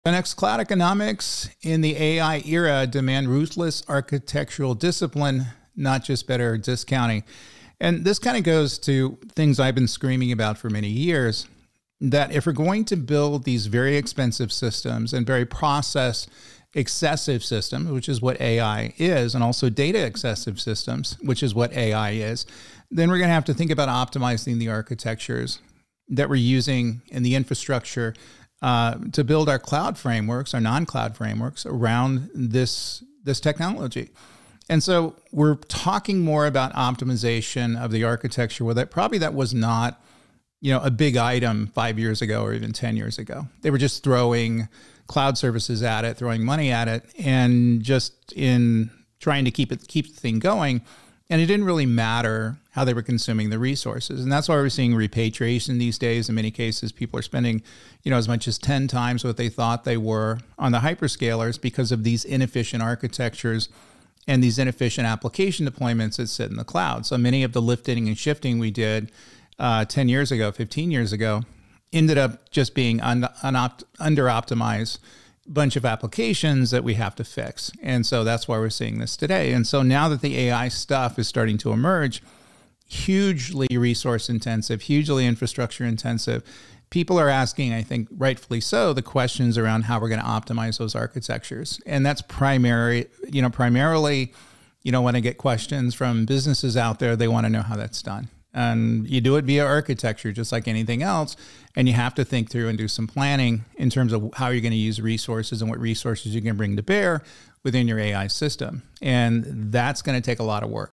AI Cloud Economics: Optimize Architectures for Cost Savings
AI's resource intensity means cloud economics now demand ruthless architectural discipline. No more throwing money; optimization is key. Inefficient architectures lead to cost explosions and repatriation.